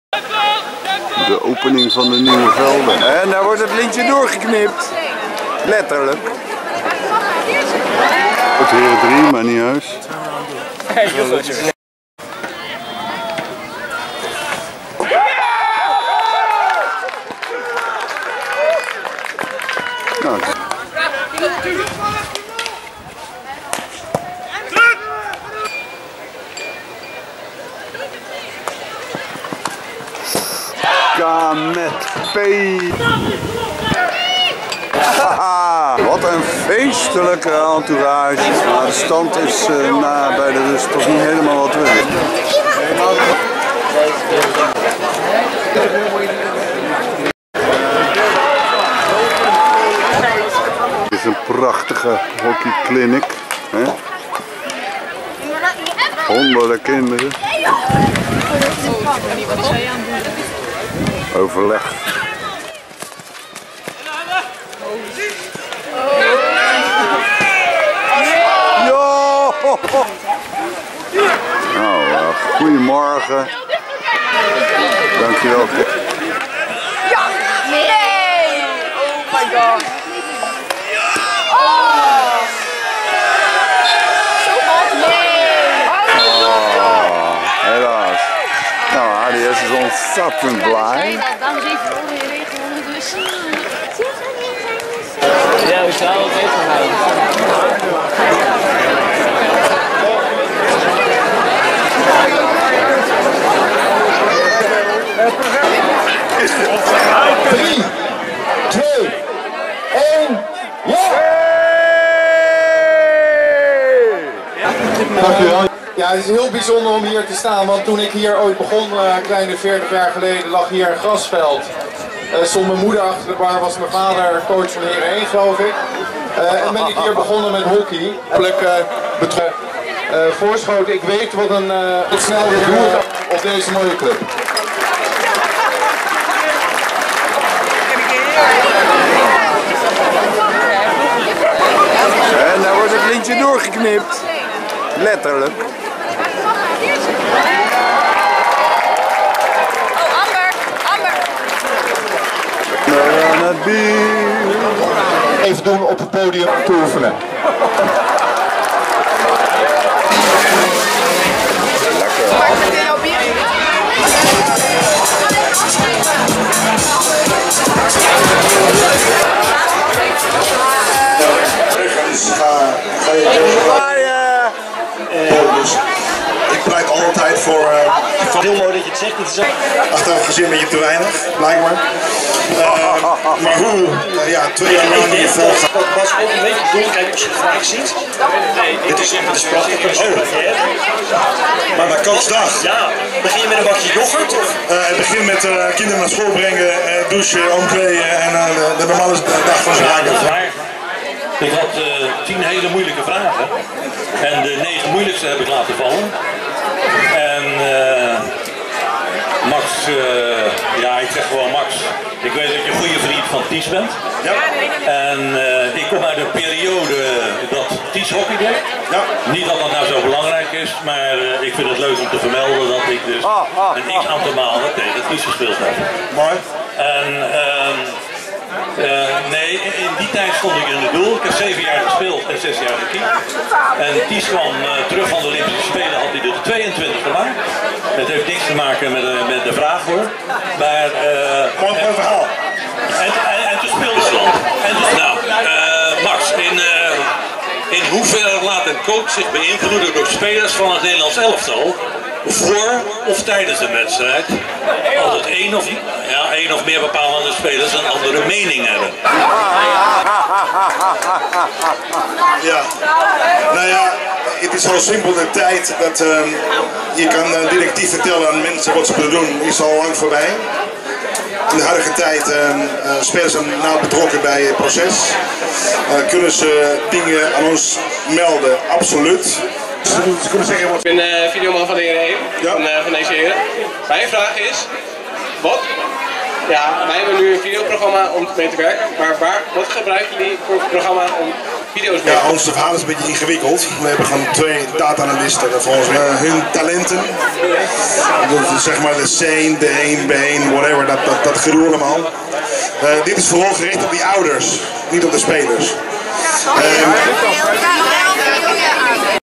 4esc 25sep16 H1 HDS feestelijke opening velden
Feestelijke inwijding van de nieuwe velden bij Forescate. Met Max Caldas. O ja, ook Heren 1 speelt...